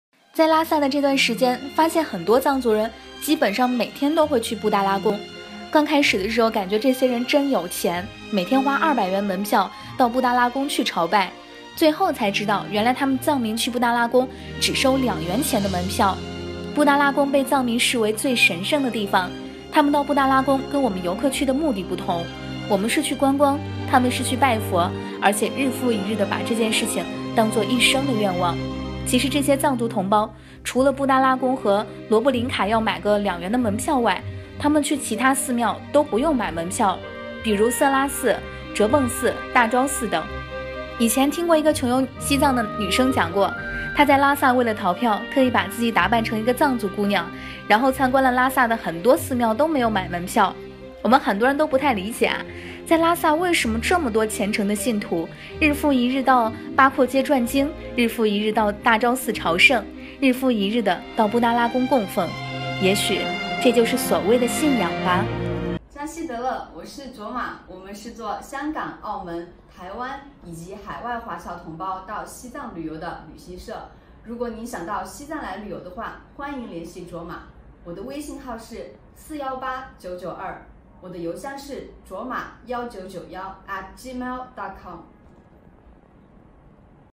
旺季的布达拉宫门票那么难买，为何藏族人却可以天天进去？
在西藏拉萨的这段时间，发现很多藏族人基本上每天都会去布达拉宫，刚开始的时候感觉这些人真有钱，每天花200元门票到布达拉宫去朝拜，最后才知道原来他们藏民去布达拉宫只收2元钱的门票。